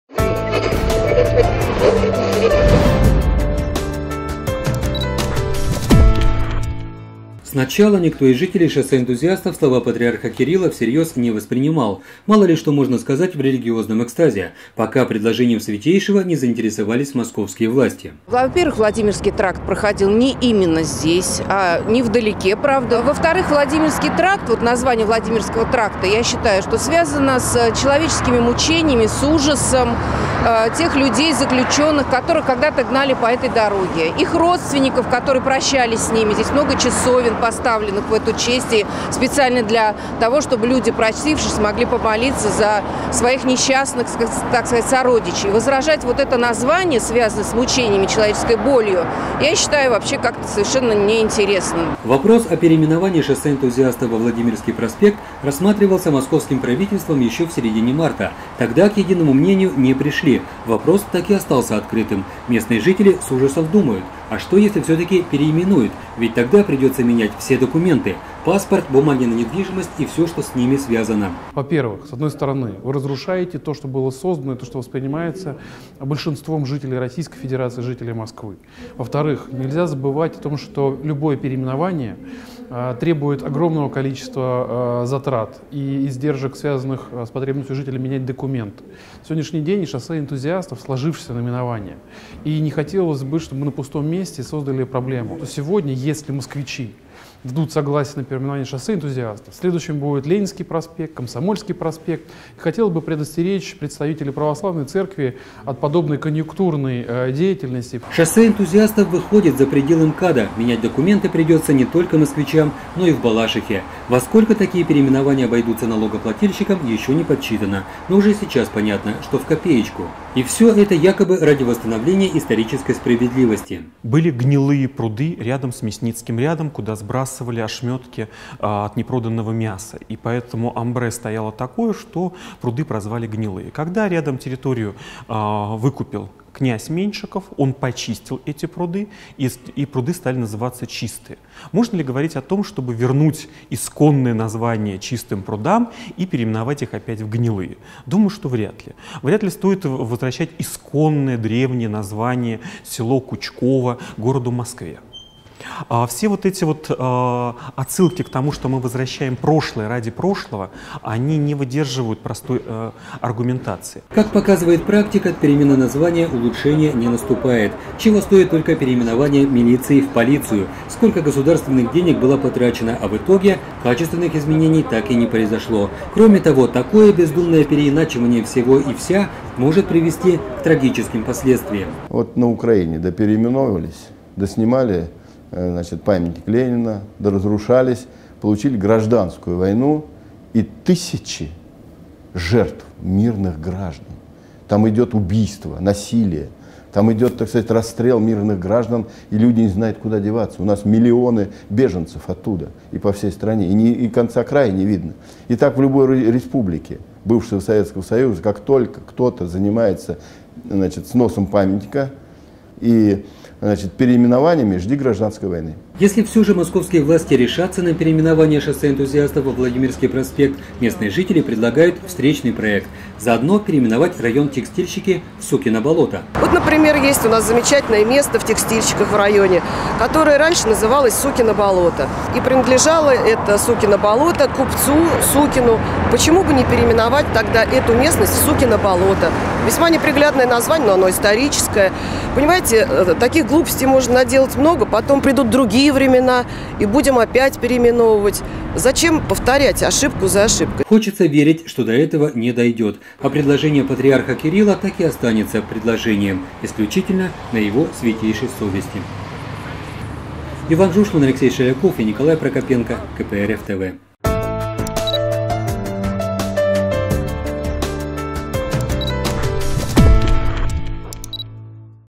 Москвичи просят переименовать Текстильщики в «Сукино болото», а Чистые пруды в «Гнилые пруды»
На такие резкие заявление людей подвергло предложение Патриарха Кирилла переименовать Шоссе Энтузиастов во Владимирский проспект. Если это произойдёт, из столичного бюджета надо будет тратить  миллионы рублей. А жители Шоссе Энтузиастов завязнут в бюрократическом болоте перемены документов. Репортаж Ивана Жушмана, Алексея Шелякова и Николая Прокопенко.